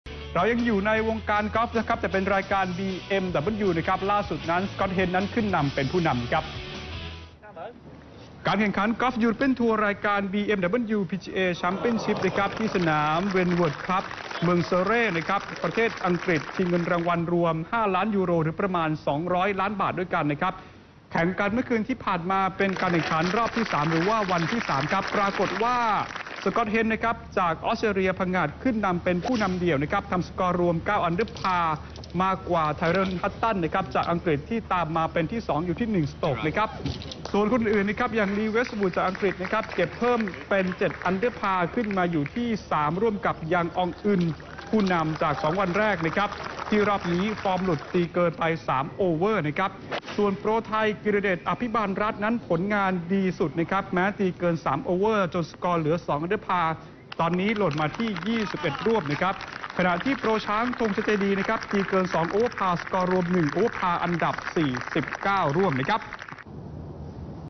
"สกอตต์ เฮนด์" ขึ้นนำเดี่ยววันที่สาม กอล์ฟ BMW แชมเปียนชิป
พิกัดกีฬา ช่วงเช้าเที่ยง : "สกอตต์ เฮนด์" ขึ้นนำเดี่ยววันที่สาม กอล์ฟบีเอ็มดับเบิลยู แชมเปียนชิป "NOW26" 29-5-59